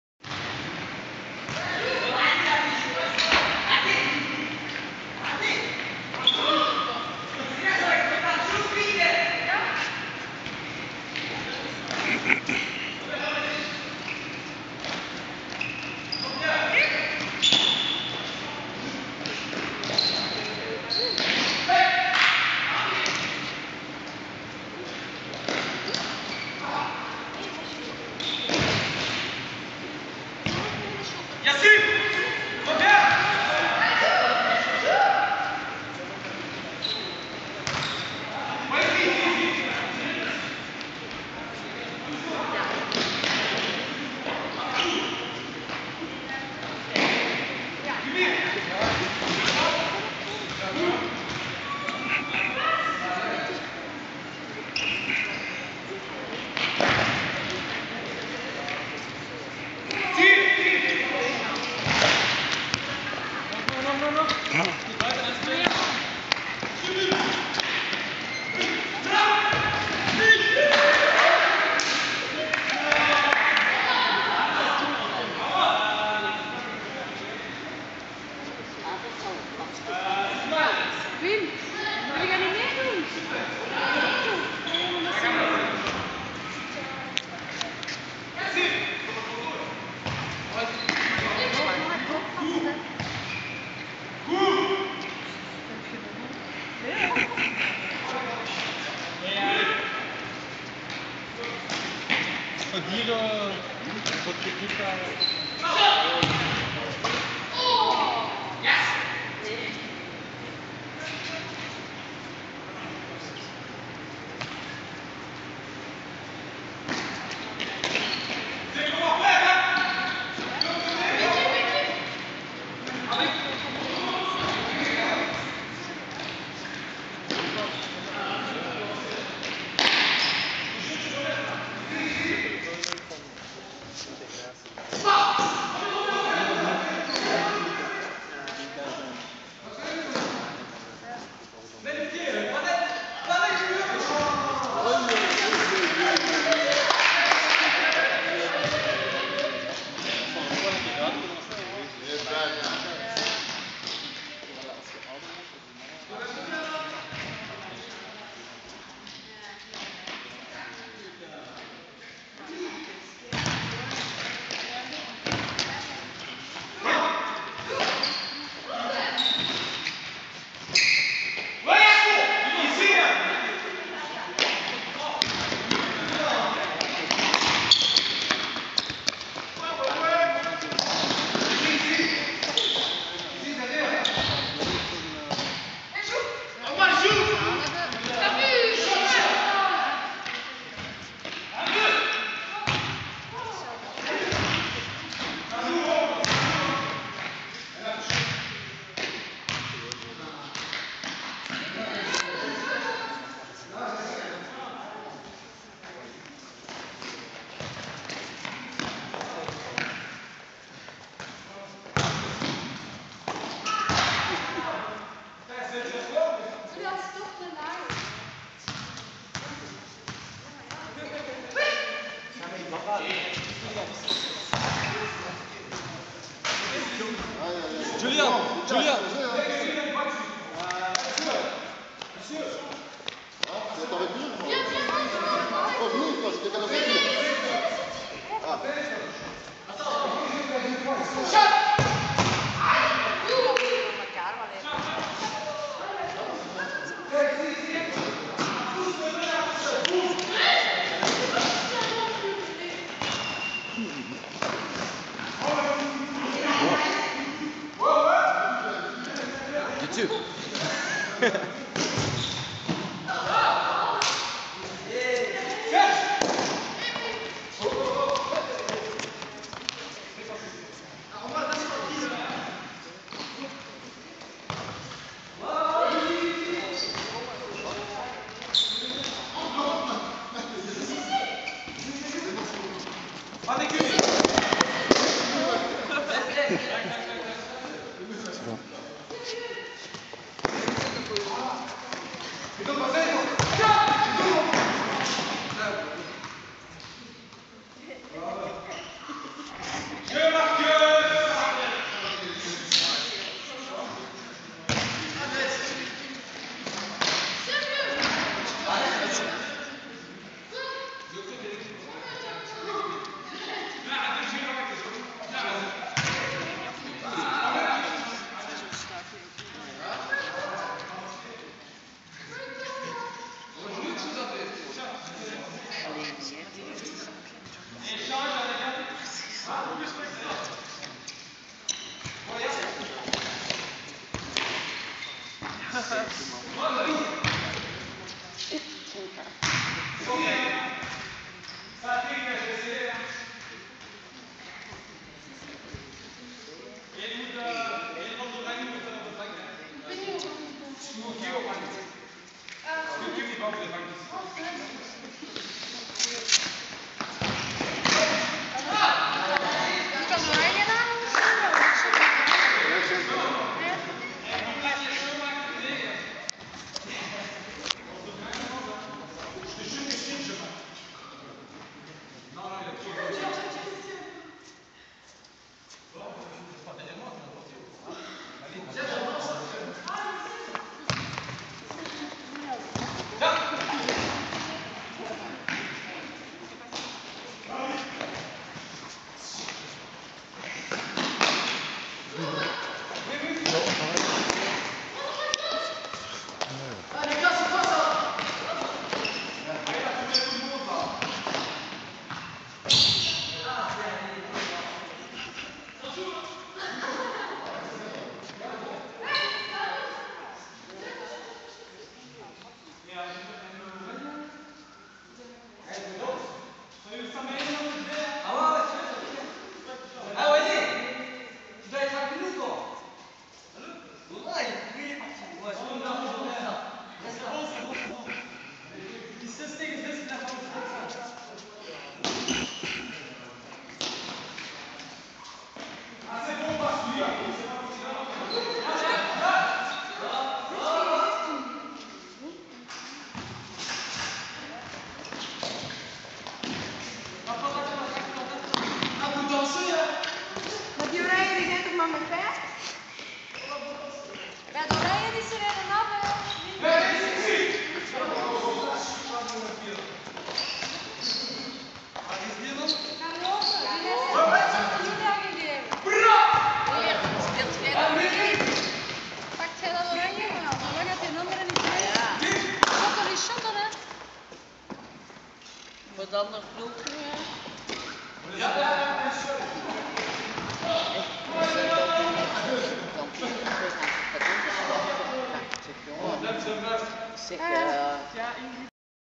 Jongeren Spelen Voetbal In Strombeek-bever (21/12/2012)
Voetbal waarbij alle jongeren tussen 12 en 27 jaar welkom zijn